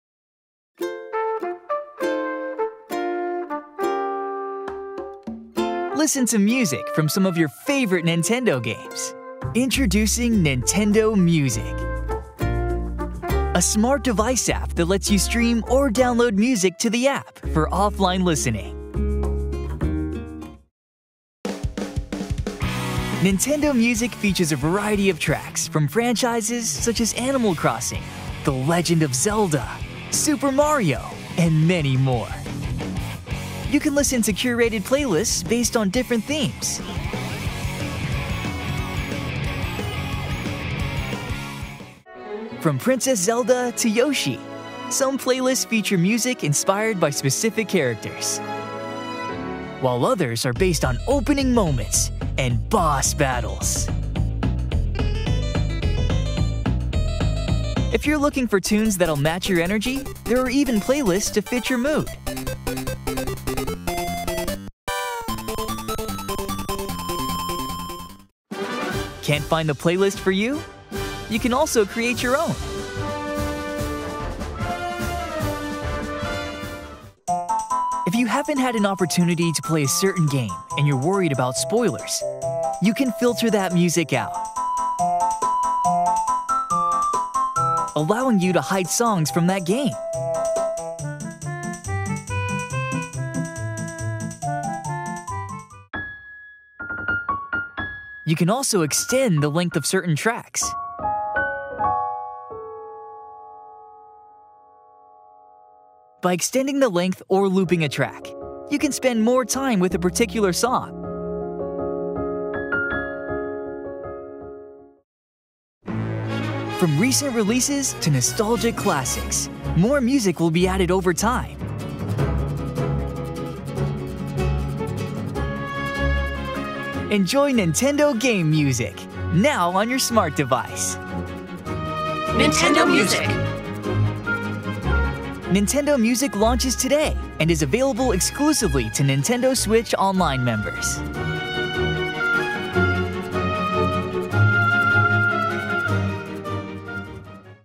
Nintendo Music – Announcement Trailer
Enjoy Nintendo game music—now on your smart device! Nintendo Music features a variety of tracks from Nintendo franchises, plus curated playlists based on characters, in-game moments, and moods. From recent releases to nostalgic classics, even more music will be added in the future.

Nintendo Music is available today, exclusively to active Nintendo Switch Online members.

Nintendo Australia is your home for the latest news, games, and updates!

Visit the official Nintendo website for all the latest products and services